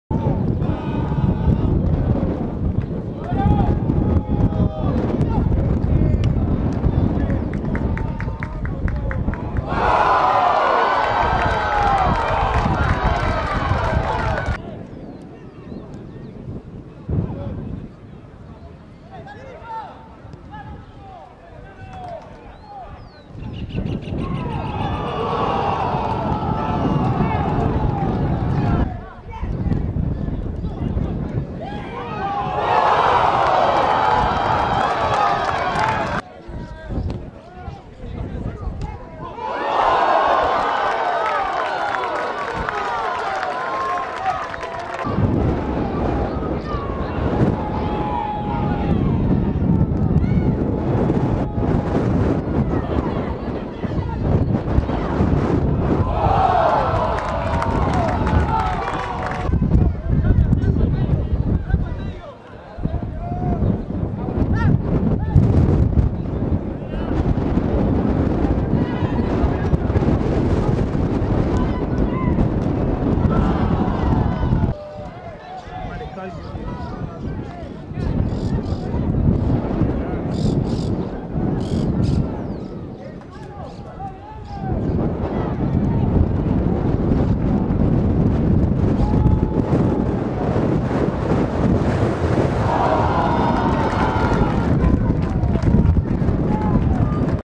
Liniers 5-2 Huracán (Fecha 10 - Liga del Sur)
Liniers goleó a Huracán por 5-2 en cancha de Libertad y sostiene la distancia de cinco unidades con su escolta. Juan Perotti, Mauro Martínez, Boris Herrera, Julio Acosta y Juan José Ramírez anotaron los goles de los "chivos". Leonel Navarro y Alan Nungeser convirtieron para el "globo".